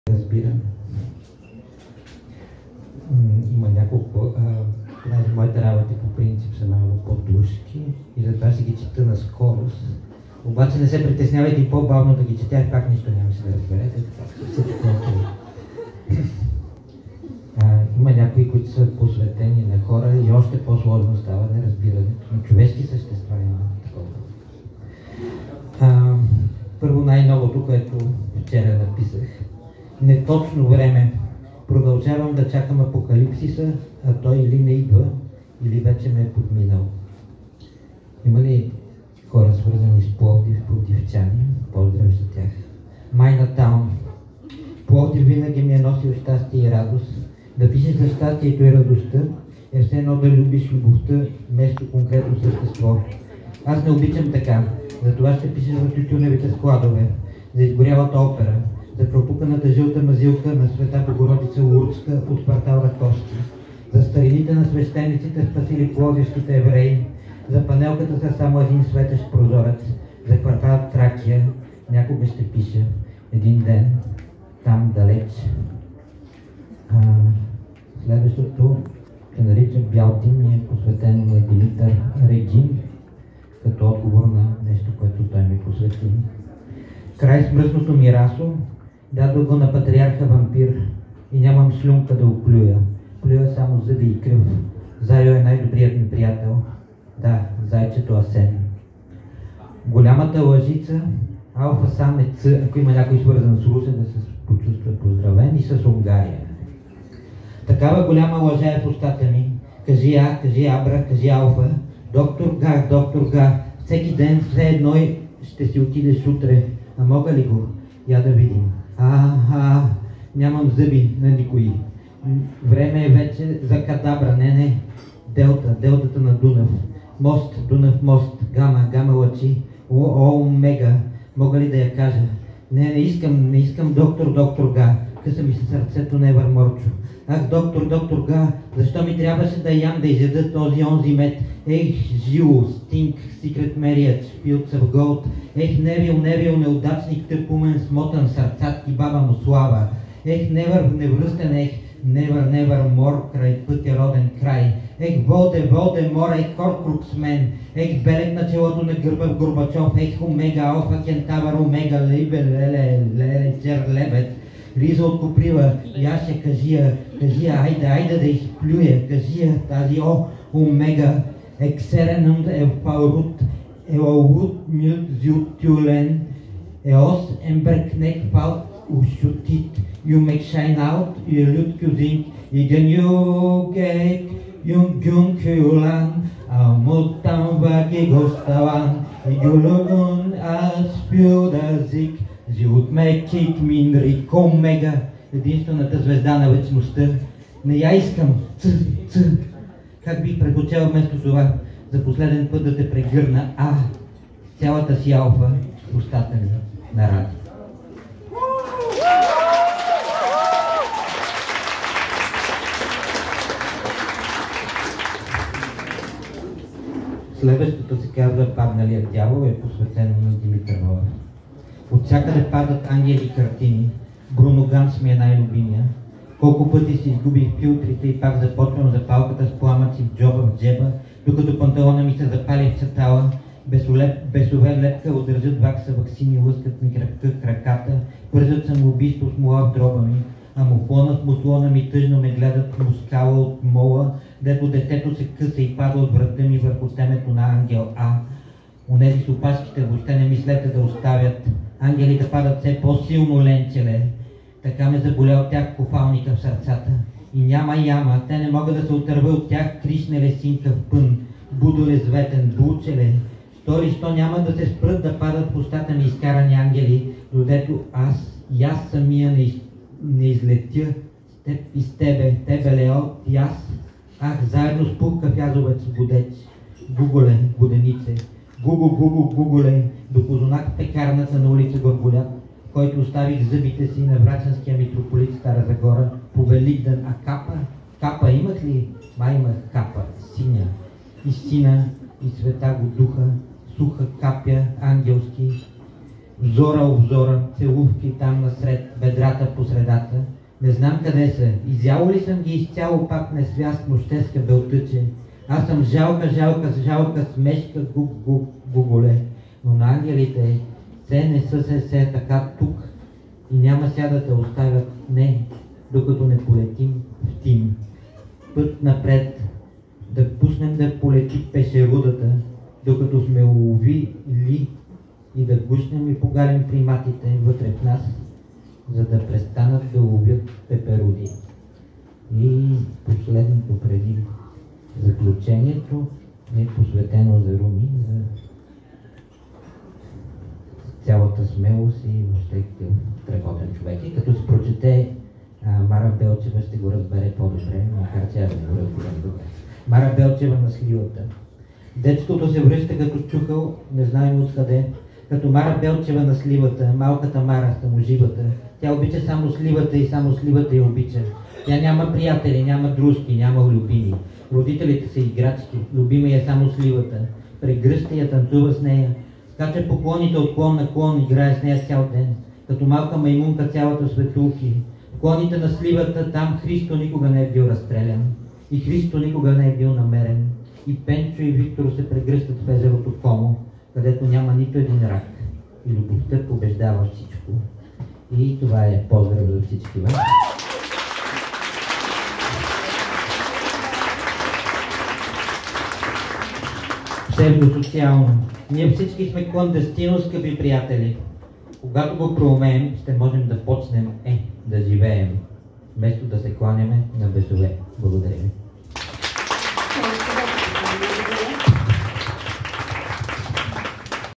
Петър Канев чете поезия с неразбиране в насп Падналият дявол
Ние всички сме clandestino, скъпи приятели!
Когато го проумеем,
ще можем да почнем е да живеем,
вместо да се кланяме
на бесове.